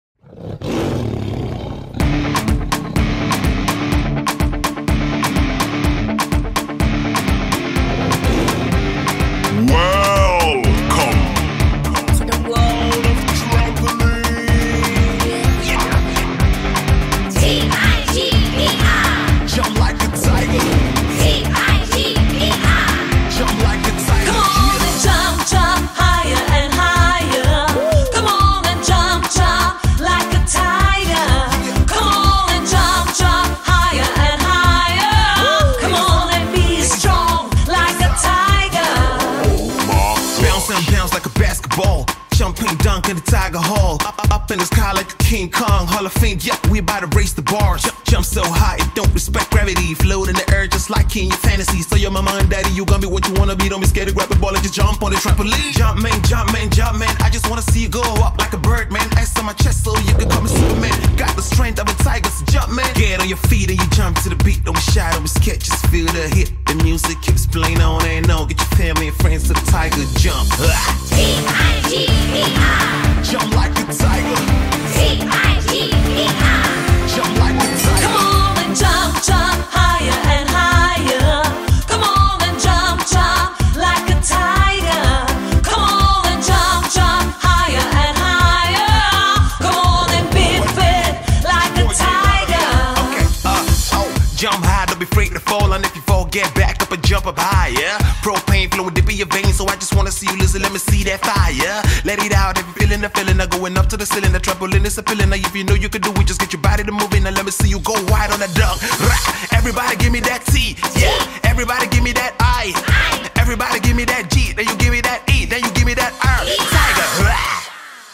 TIGER JUMP Trampolinpark in Oberhausen
Der größte Trampolinpark Deutschlands! - TIGER JUMP Trampolinpark am CentrO in Oberhausen!

3.500 qm Event- u. Actionfläche - Über 100 Trampoline, Pro Jump, Tower Jump, Dodgeball - BECOME STRONG LIKE A TIGER

Reservierungen für Gruppen unter 10 pers. nicht notwendig - Komm auf einen Sprung vorbei!

Vielen Dank an "Sky Motion" aus Oberhausen für das Videomaterial!